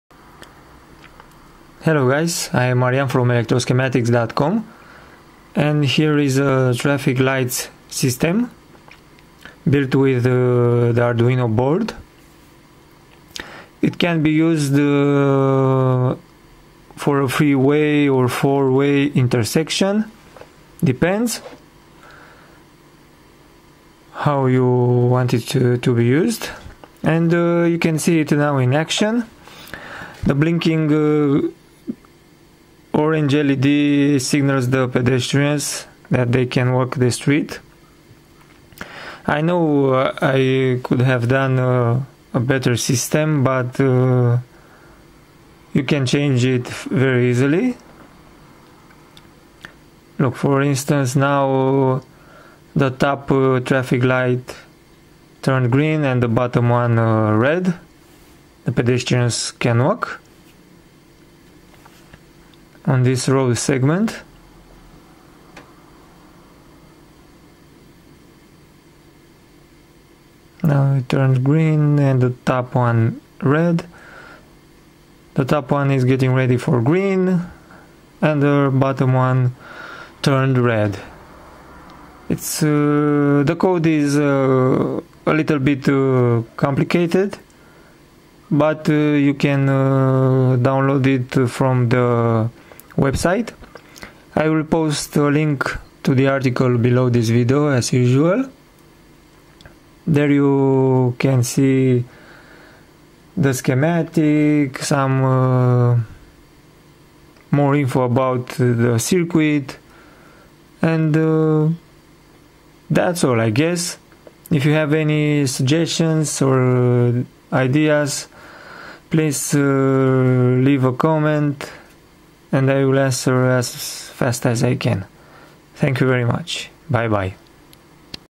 Arduino Traffic Lights System
Here is a traffic light system based on Arduino that can be use in a 3-way ond 4-way interections.